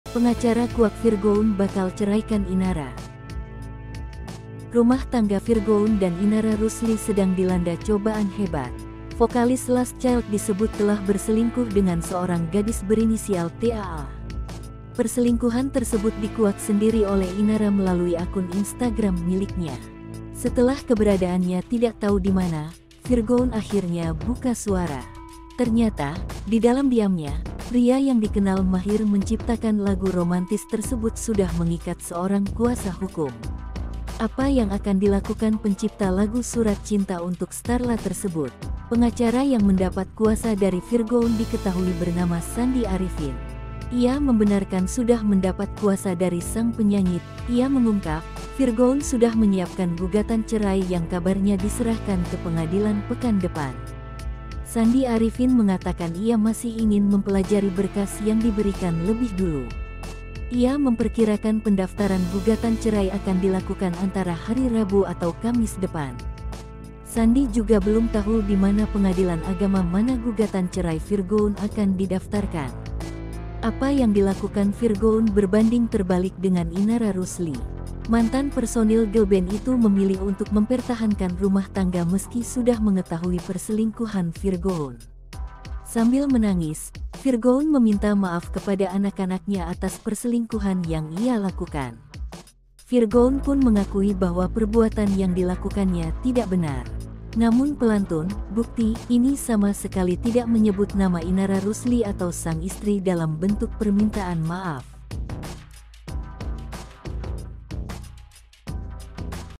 Pengacara Kuak Virgoun Bakal Ceraikan Inara
Rumah tangga Virgoun dan Inara Rusli sedang dilanda cobaan hebat.

Vokalis Last Child disebut telah berselingkuh dengan seorang gadis berinisial TAA.

Perselingkuhan tersebut dikuak sendiri oleh Inara melalui akun Instagram miliknya.

Berita Selengkapnya :

Yuk lihat informasi terkini bersama kami, di:

Palembang.tribunnews.com

Editor: Bima
Sumber: ig/virgoun_, ig/mommy_starla, ig/sandyarifinsh